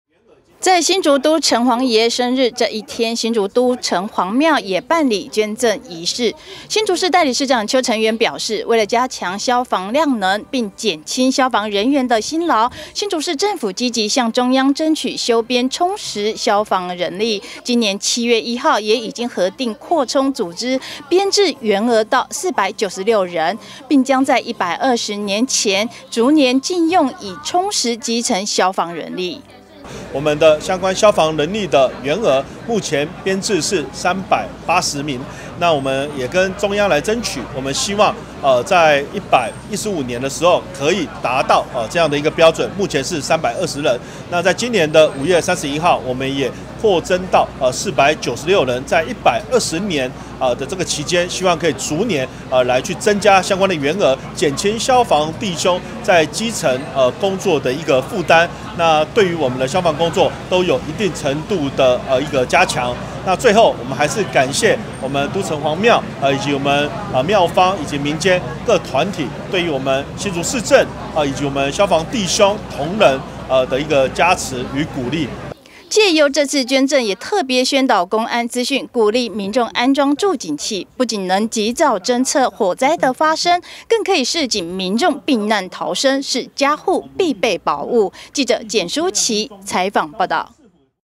減輕消防負擔 竹市逐年充實基層消防人力
為了加強消防量能、並減輕消防人員辛勞，新竹市政府積極向中央爭取修編充實消防人力，今年7/1也已經核定擴充組織編制員額到496人，並將在120年前，逐年進用以充實基層消防人力。